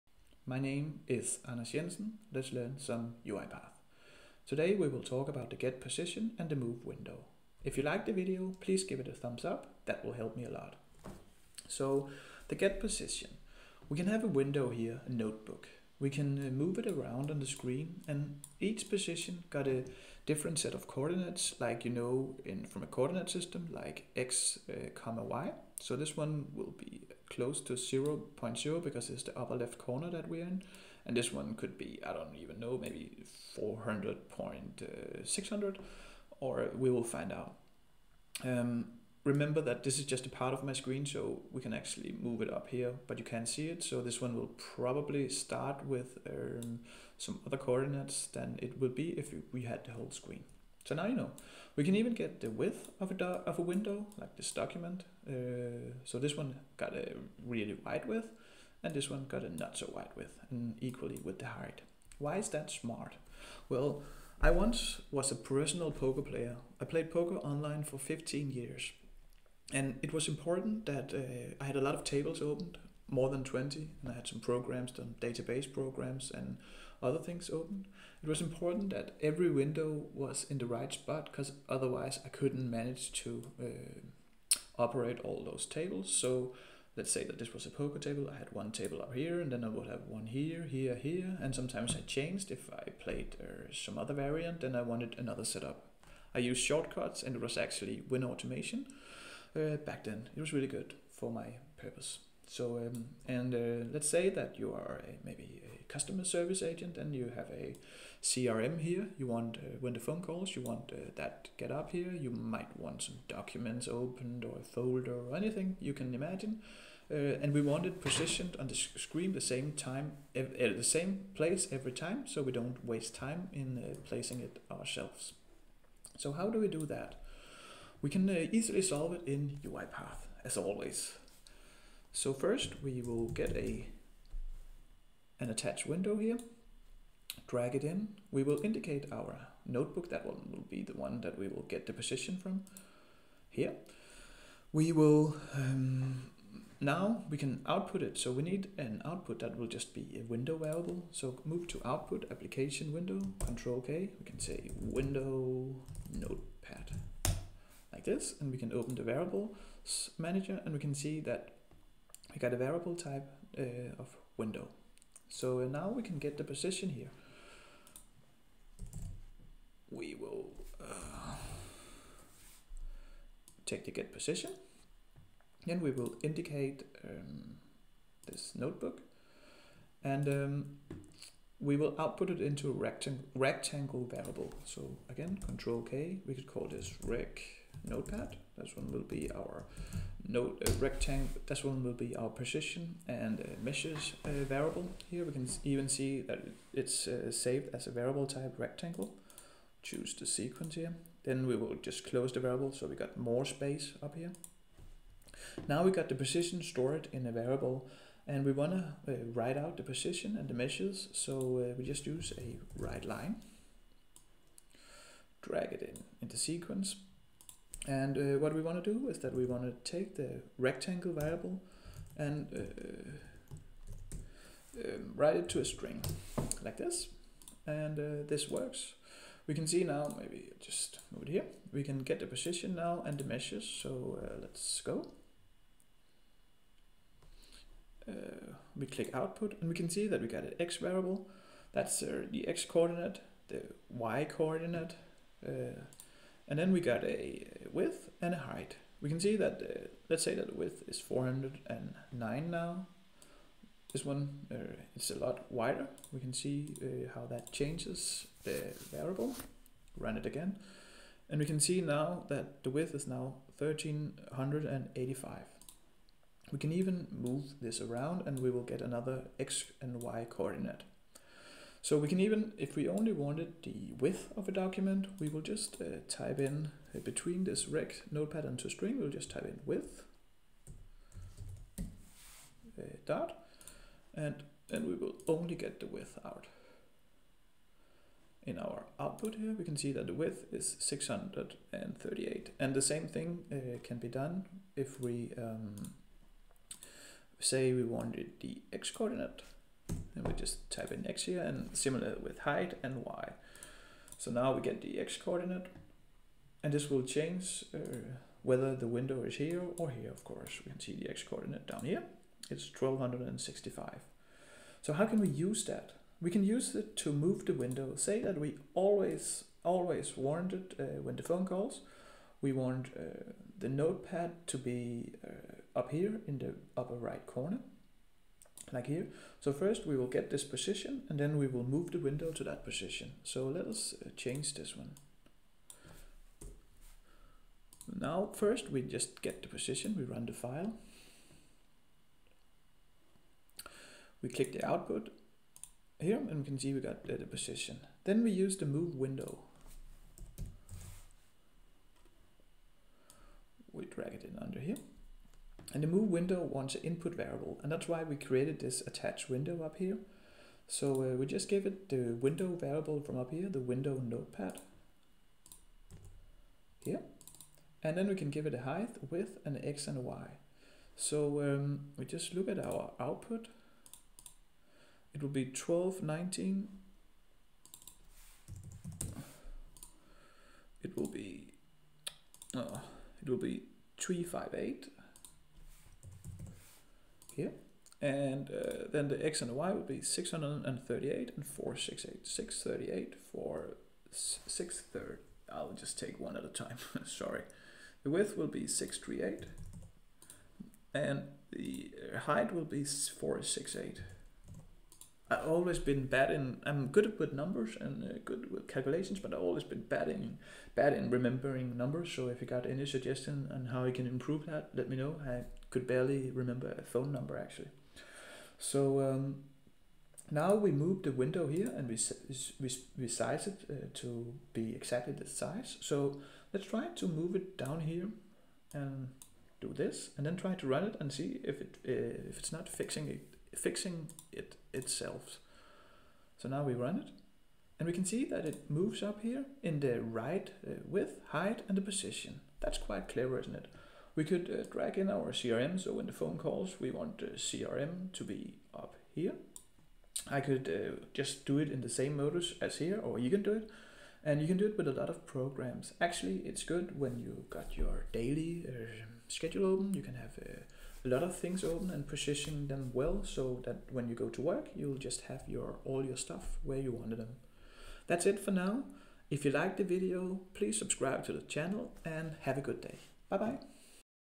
UiPath | Get Position and Move Window | Tutorial
In this guide, you'll learn how to use the UiPath activity Get Position to get the height, the width, the x-coordinate and the y-coordinate. The information found in Get Position will be used in the Move Window activity, so Windows can be moved around and scaled with UiPath.

02:13 Attach Window
02:33 Output to a Window variable
02:49 Get Position
03:05 Create a Rectangle variable
03:33 Write out the Coordinates, Height and Width of a window.
05:40 Move Window